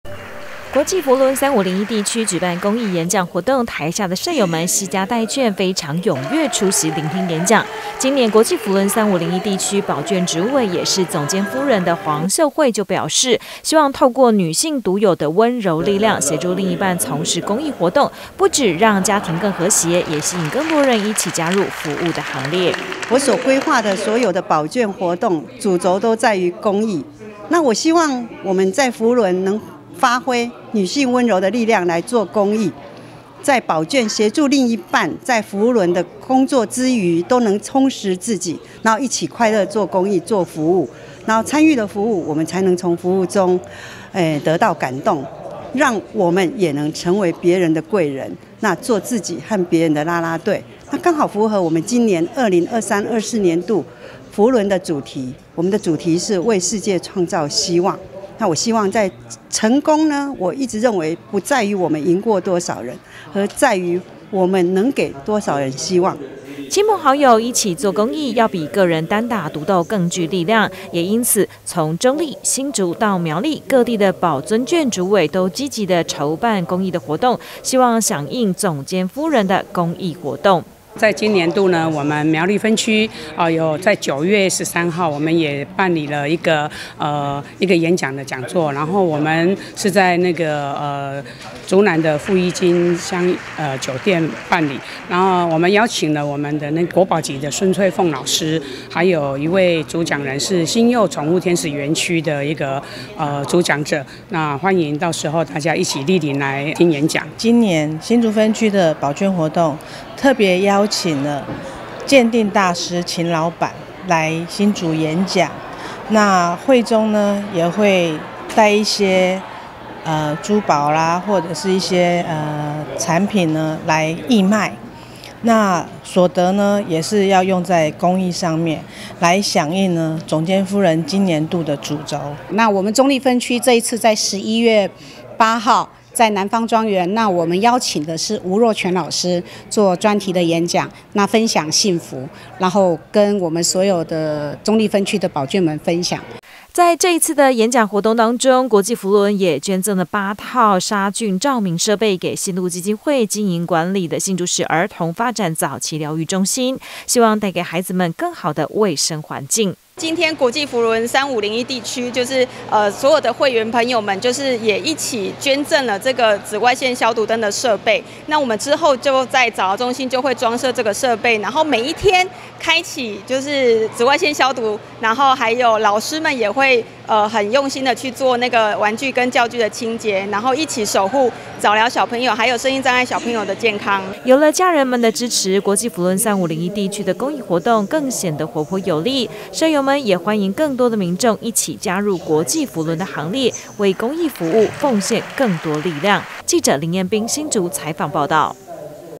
寶眷支持與陪伴 國際扶輪公益行列更有力
很多國際扶輪的社友們，出席活動都是攜家帶眷，甚至也邀請親朋好友一起參加，有了家人們的支持，社友們可以獲得更多的力量，在公益服務的行列，投入的人數也就更多了。今年國際扶輪3501地區的寶眷主委們，就準備籌辦一系列的活動，希望讓社友們可以一起服務公益，一起學習、一起成長。